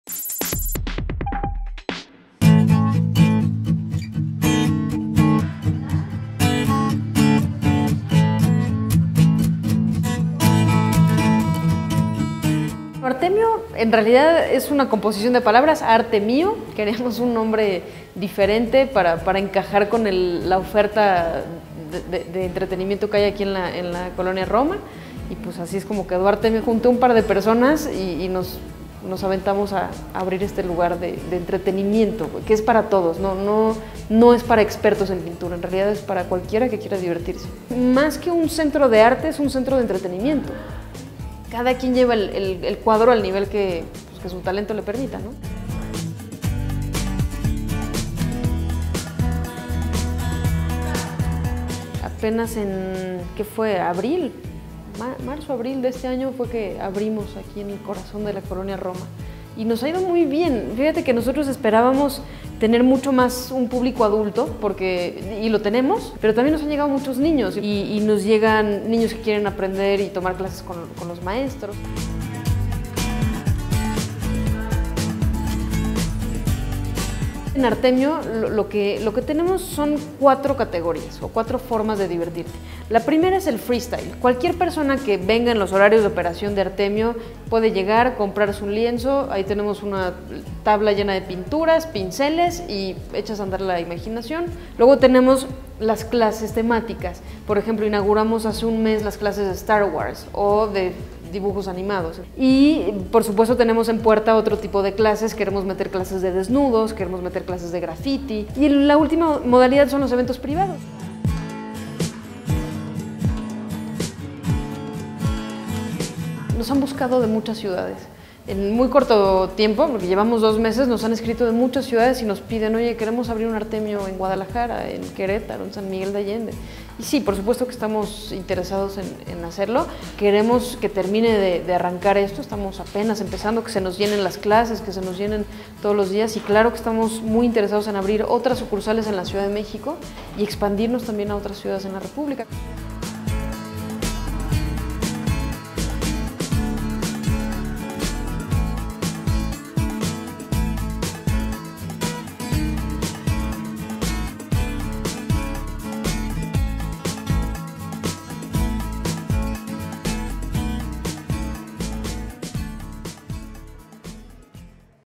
Fundación Artemio, arte al alcance de todo el público
Se trata de una nueva oferta cultural ubicada en el corazón de la Colonia Roma que busca expandir la oferta de entretenimiento cultural de la Ciudad de México